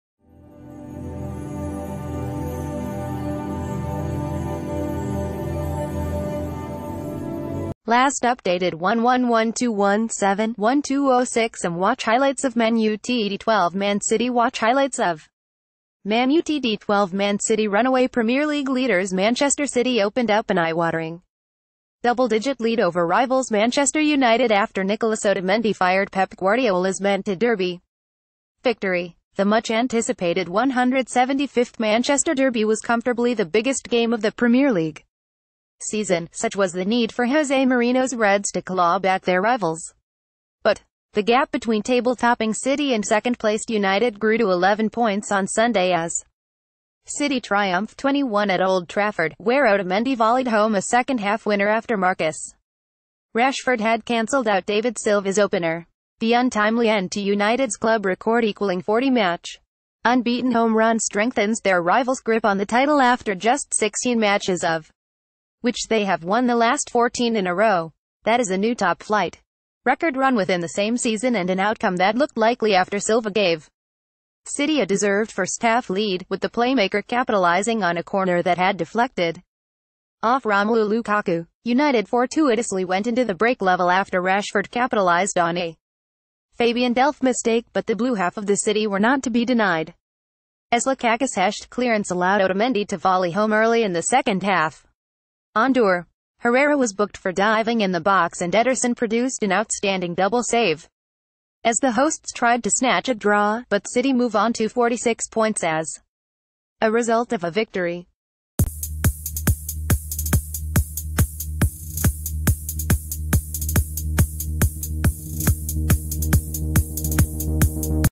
Watch: manchester united 1-2 manchester city highlights
Last Updated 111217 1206am Watch highlights of Man Utd 12 Man City Watch highlights of Man Utd 12 Man City Runaway Premier League leaders Manchester City opened up an eyewatering doubledigit lead over...